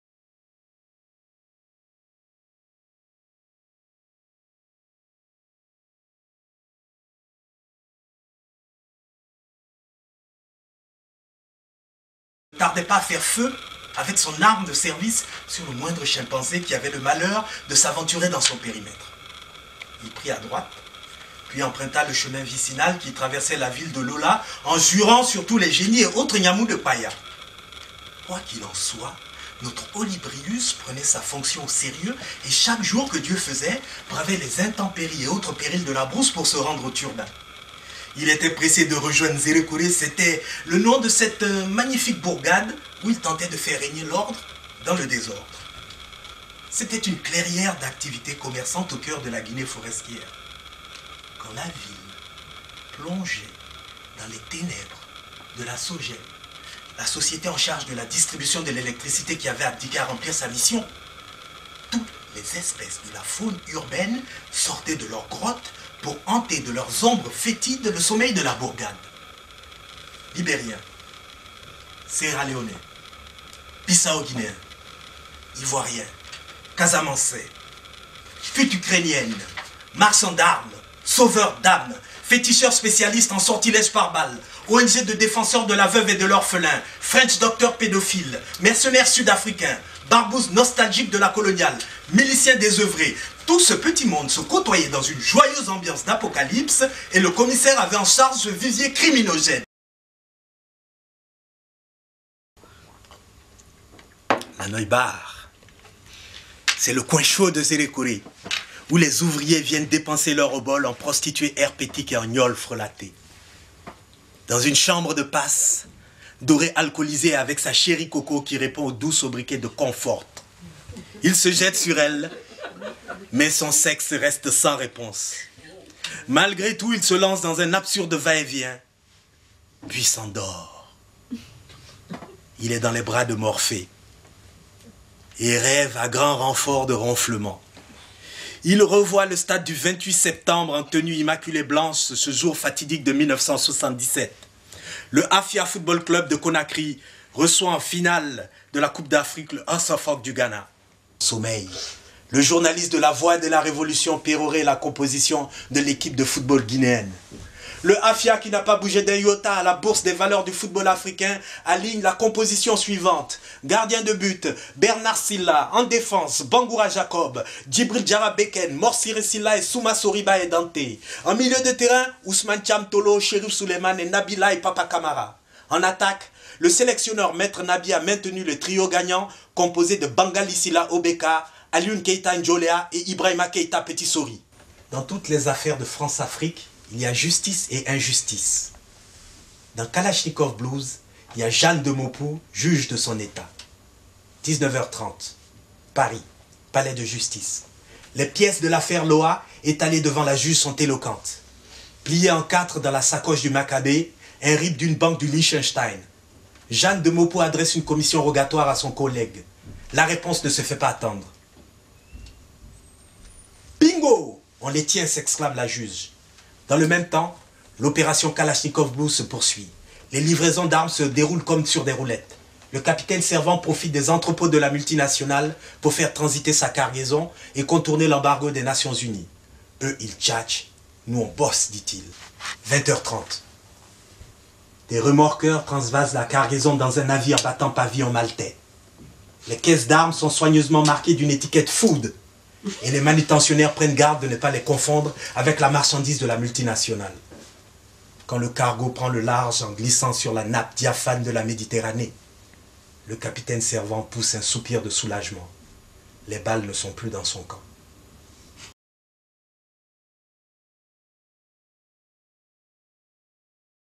Sunjata Koly - Lecture kalachnikov blues
Lecture de l'adaptation théâtrale de Kalachnikov blues par Sunjata. La mise en lecture a été réalisée par Sebastien Lagord (Compagnie Astrolabe).
Kalachnikov Blues est une intrigue policière  qui se déroule Paris et N'zérékoré une ville de la Guinée forestière où le commissaire Doré tente de faire régner l'ordre dans le désordre. Il doit faire face aux réseaux mafieux de la françafrique.